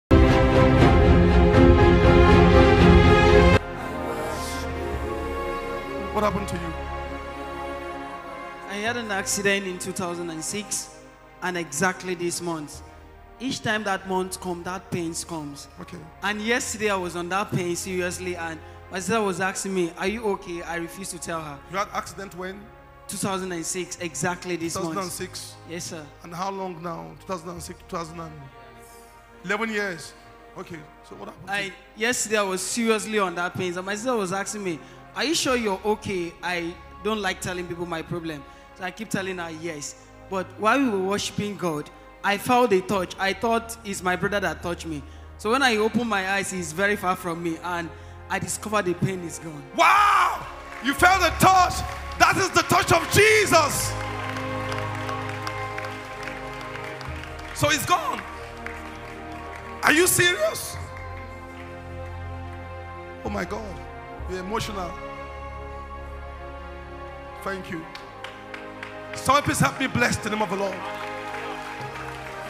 HEALING TESTIMONIES, FROM THE HEALING STREAM @RCCG THRONE ROOM
HEALING STREAMS.
Special Healing Service
@RCCG Throne Room
on 22nd October 2017
@ The Congress Hall, Transcorp Hilton,
Abuja.